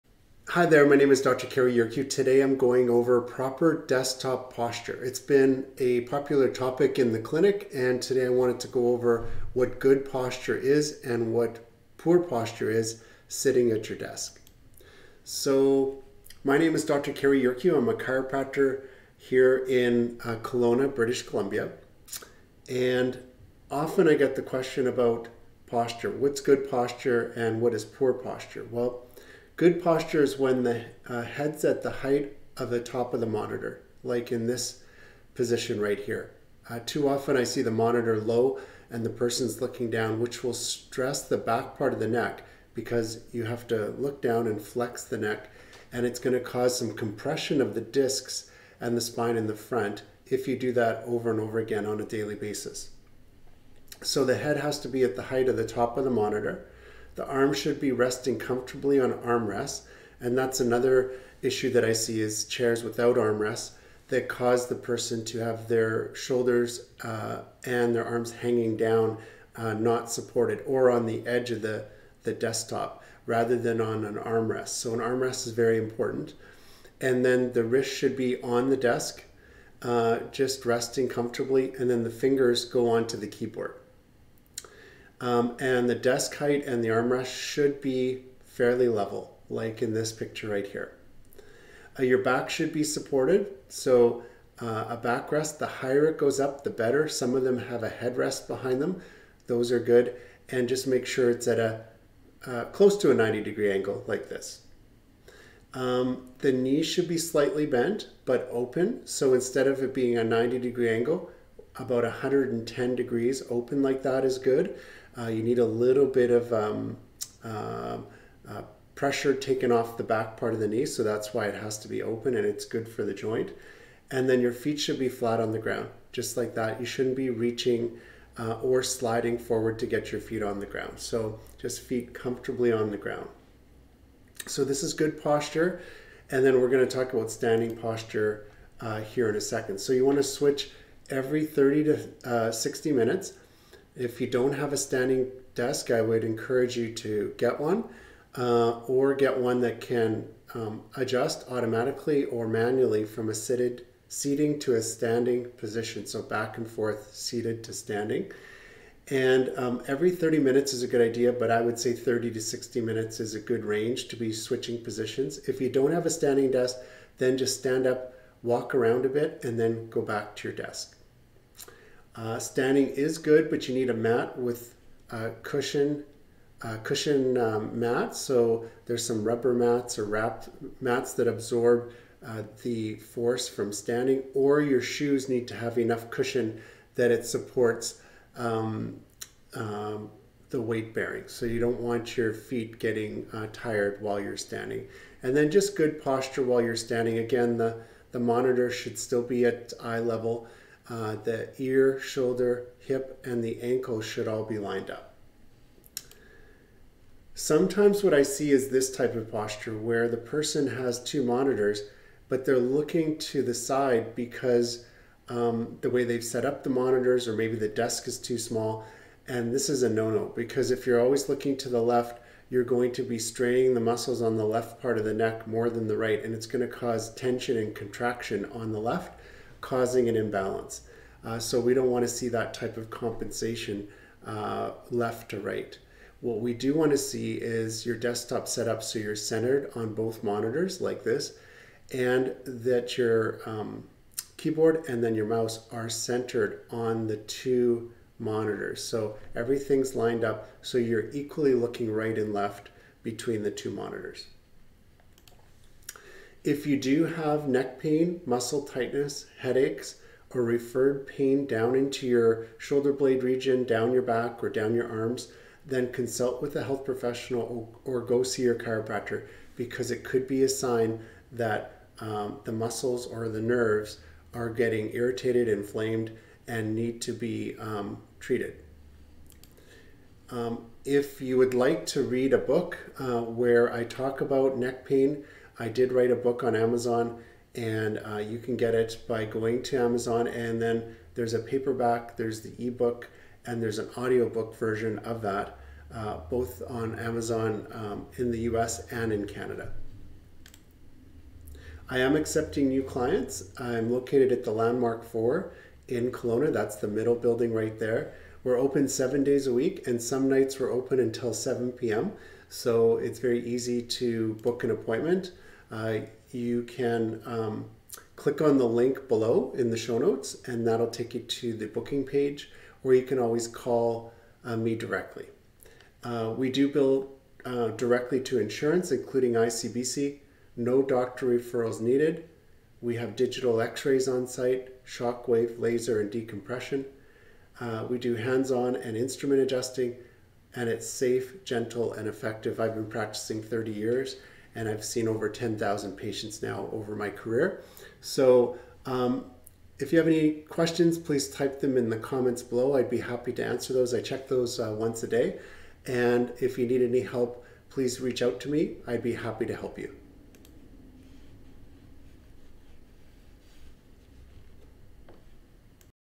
Good desktop posture vs poor posture
What is good desktop posture vs poor posture?   Learn more in this video by Dr. Cary Yurkiw, Chiropractor in Kelowna.